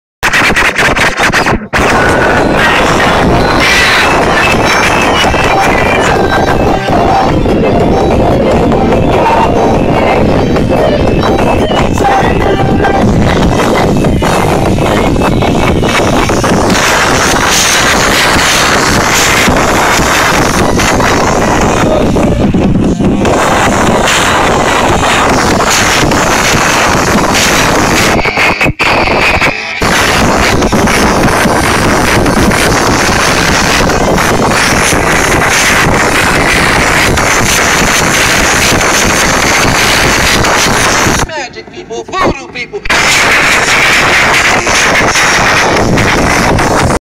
Rdlc Rave de los comidos 2009
De mañaneo en la Rave de los comidos 2009 del colevtivo dj RDLC  en el Romeral